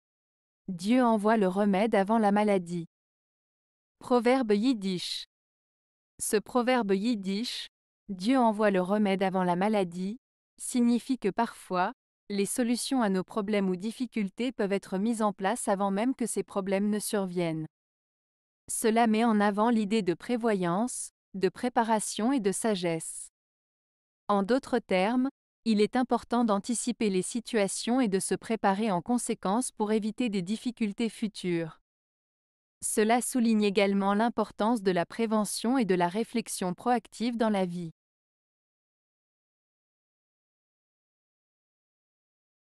Proverbe Yiddish - Dieu envoie le remède avant la maladie.
Ce proverbe Yiddish "Dieu envoie le remède avant la maladie" signifie que parfois, les solutions à nos problèmes ou difficultés peuvent être mises en place avant même que ces problèmes ne surviennent. Cela met en avant l'idée de prévoyance, de préparation et de sagesse. En d'autres termes, il est important d'anticiper les situations et de se préparer en conséquence pour éviter des difficultés futures. Cela souligne également l'importance de la prévention et de la réflexion proactive dans la vie.